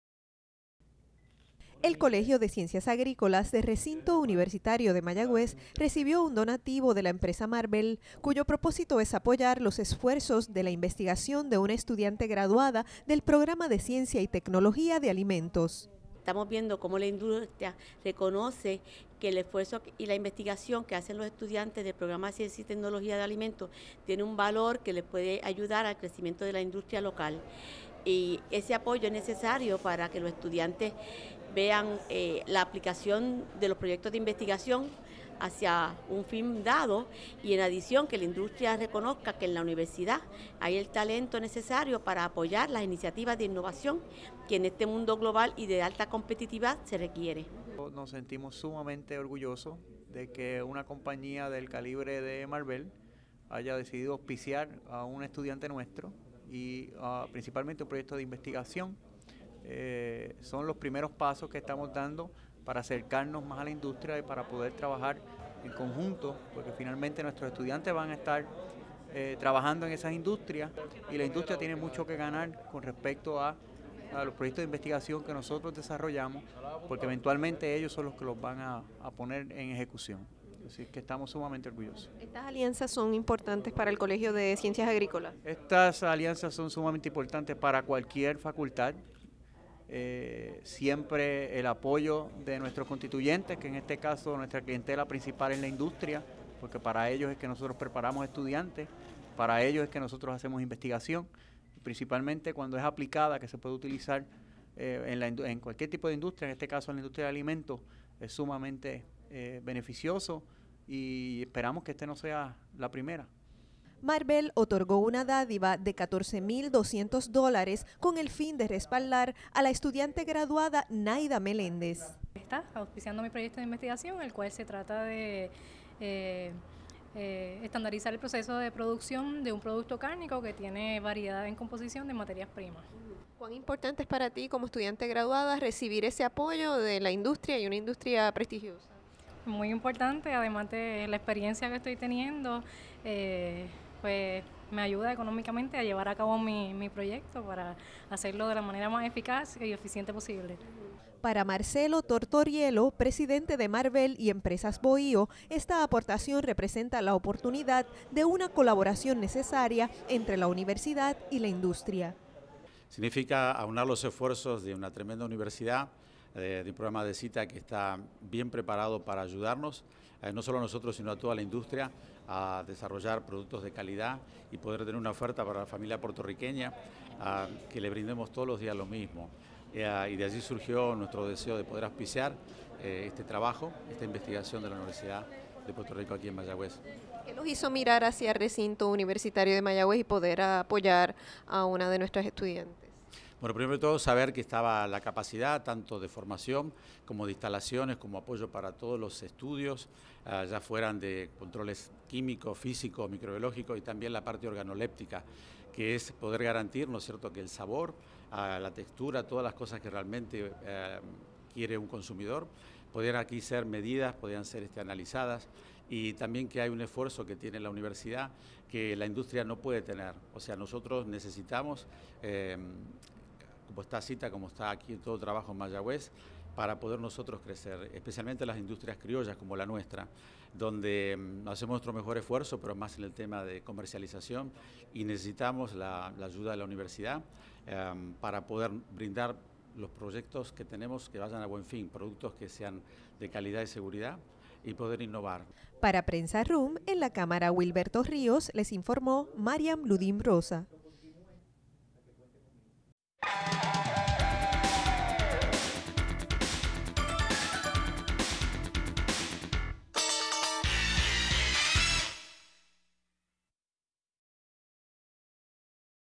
RUM recibe el apoyo de Marvel
El Colegio de Ciencias Agrícolas del Recinto Universitario de Mayagüez (RUM), recibió un donativo de la empresa Marvel, cuyo propósito es apoyar los esfuerzos de investigación de una estudiante graduada del Programa de Ciencia y Tecnología de Alimentos (CITA). Videoreportaje producido por la Oficina de Prensa. Reporta: Mariam Ludim Rosa